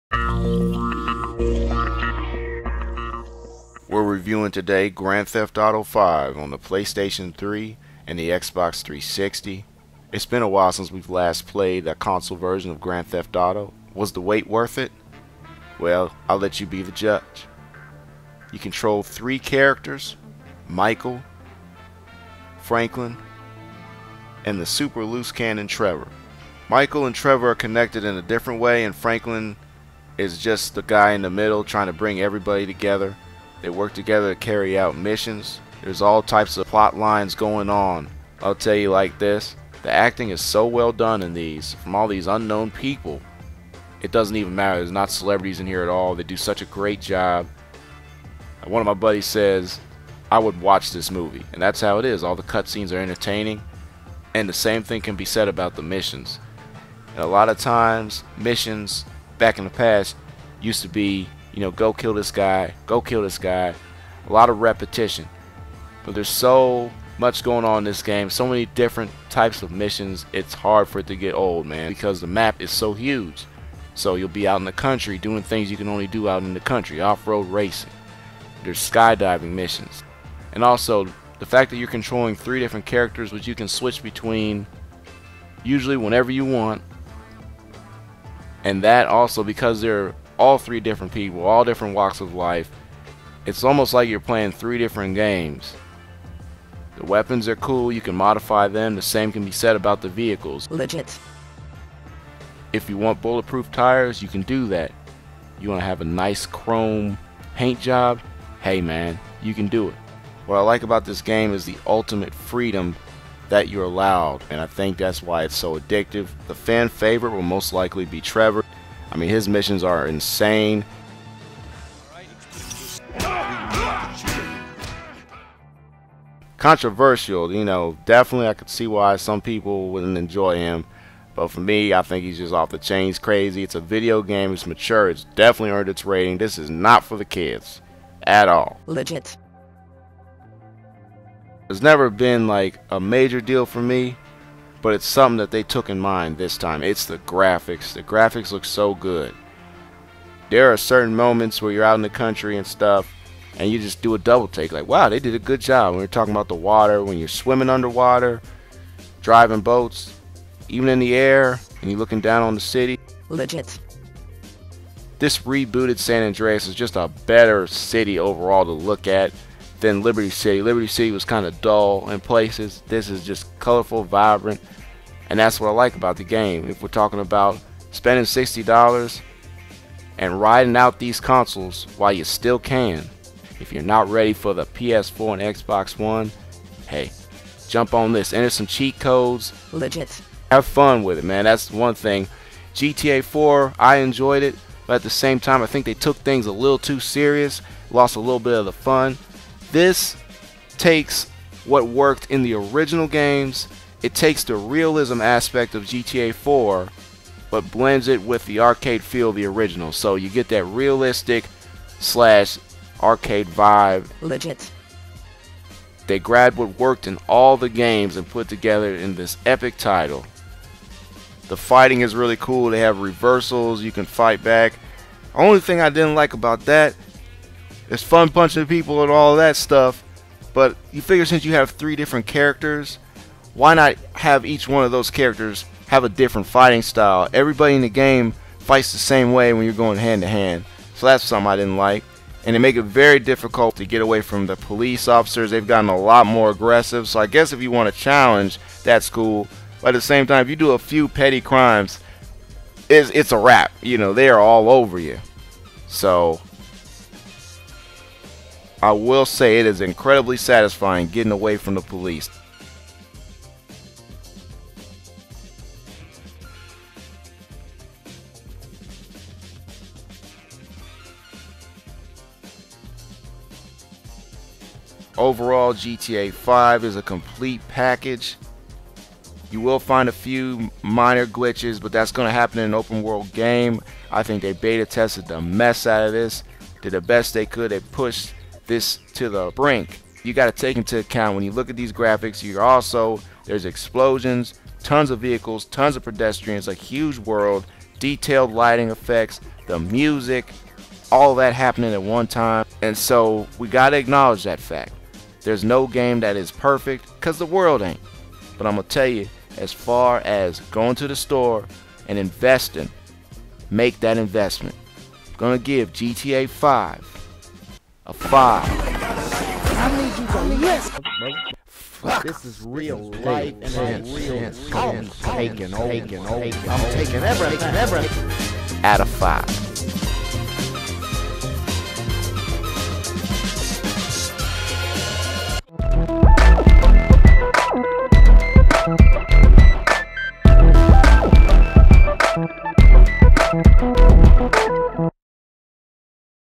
Grand Theft Auto V Review
It's been 5 Years in the Making. We travel back to Los Santos. The stakes have been raised. Gamers now control three main characters. KJ Reviews the PS3 and Xbox 360 versions of the game. Is this the greatest GTA of all time? Better than Grand Theft Auto: San Andreas (PS2, XBOX)? Your thoughts. GTA Online Review coming soon.

GTA Online